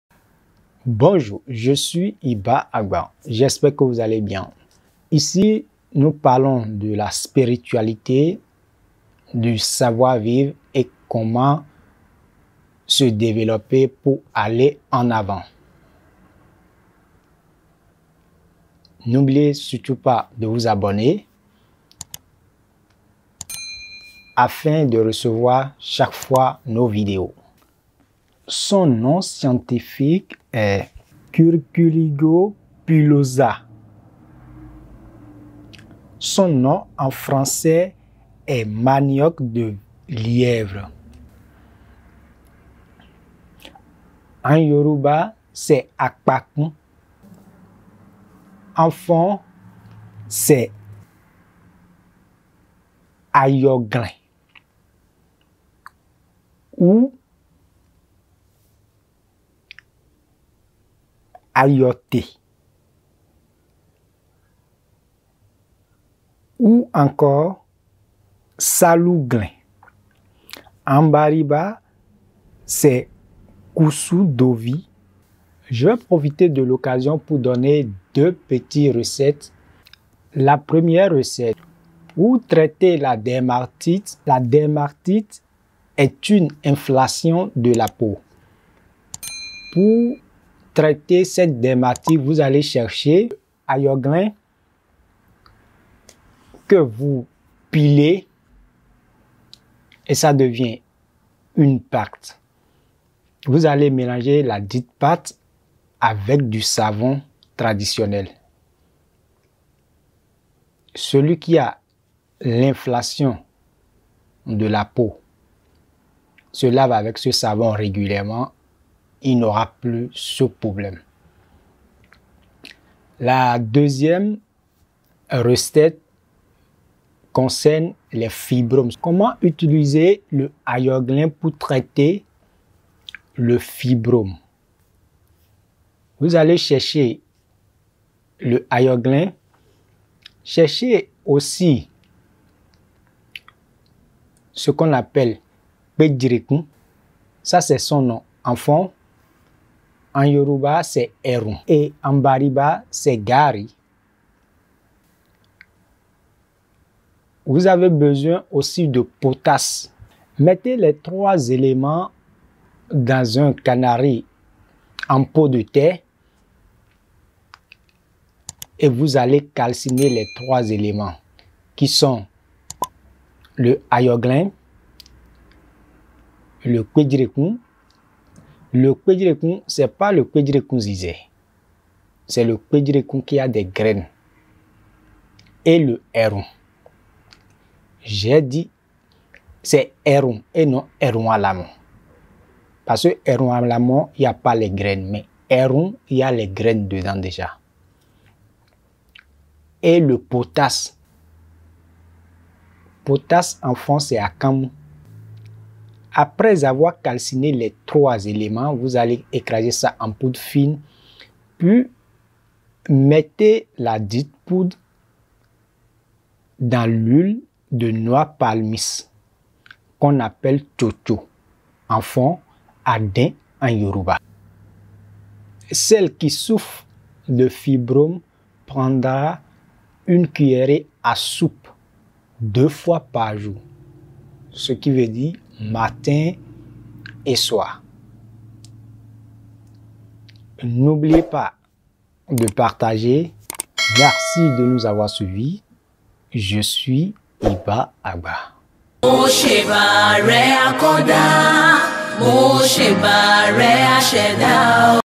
comment soigner le fibrome et la dermatite (Inflammation de la peau)
Cette vidéo, vous permettra de connaitre les différents noms de Curculigo pilosa et aussi comment soigner le fibrome et la dermatite (Inflammation de la peau) avec les fruits.
Très efficace.
Écoutez jusqu'à la fin et faites un geste d'amour en  partageant afin de sauver d'aider autres personnes qui sont dans le besoin.
Bon visionnage
Merci

IBA AGBA